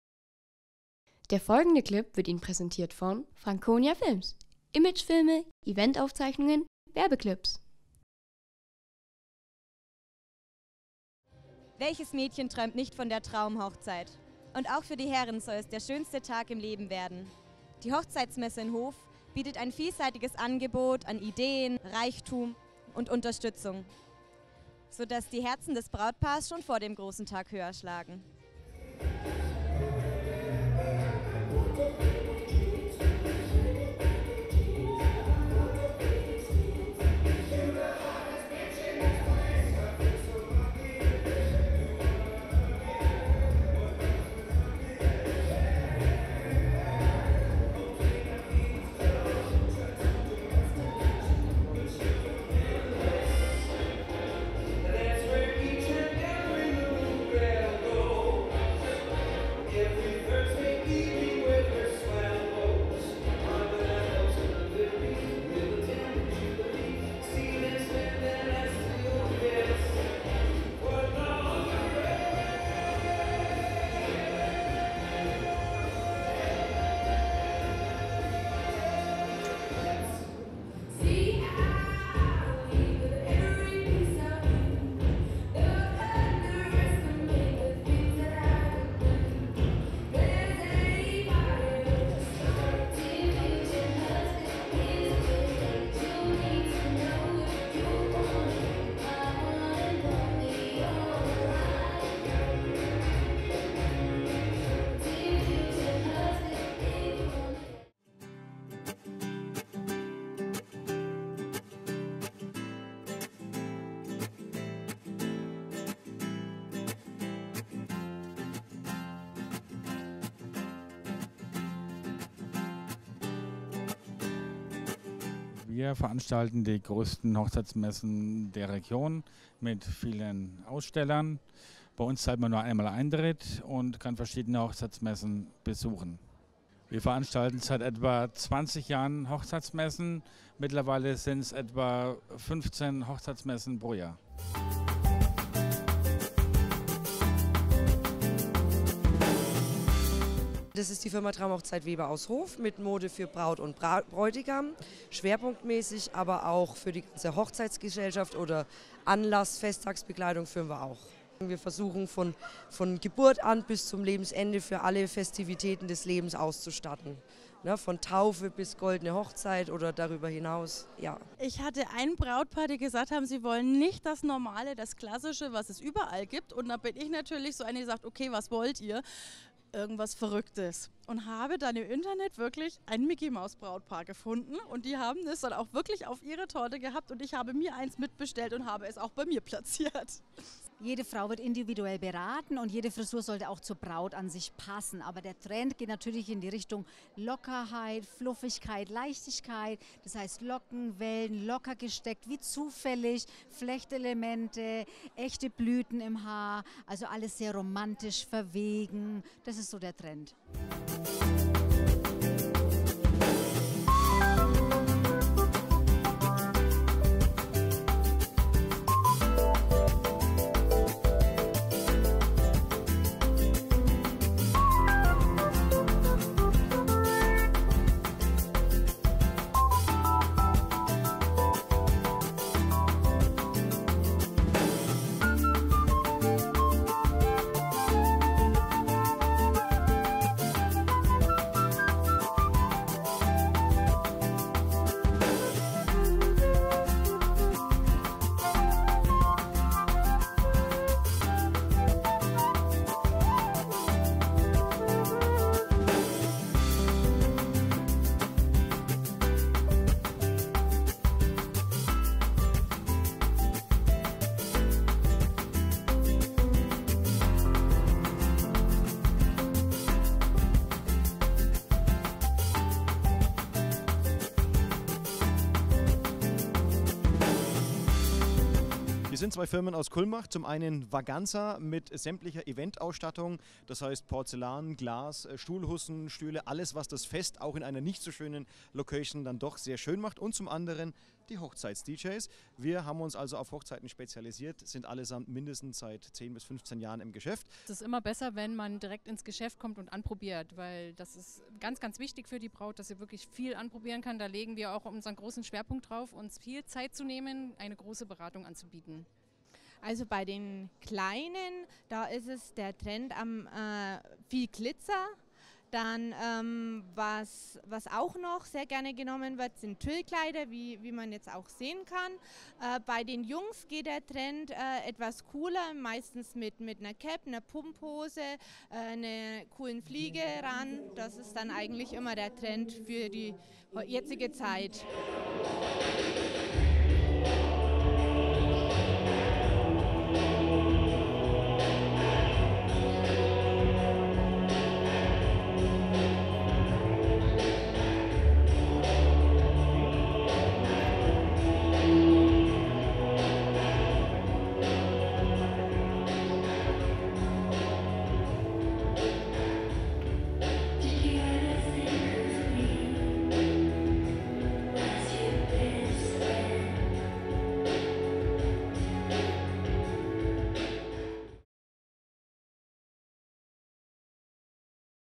Just married Hochzeitsmesse in Hof
Alle Heiratswilligen konnten sich auf der Just Married Hochzeitsmesse in Hof über Angebote informieren und mit den Planungen der Hochzeit starten.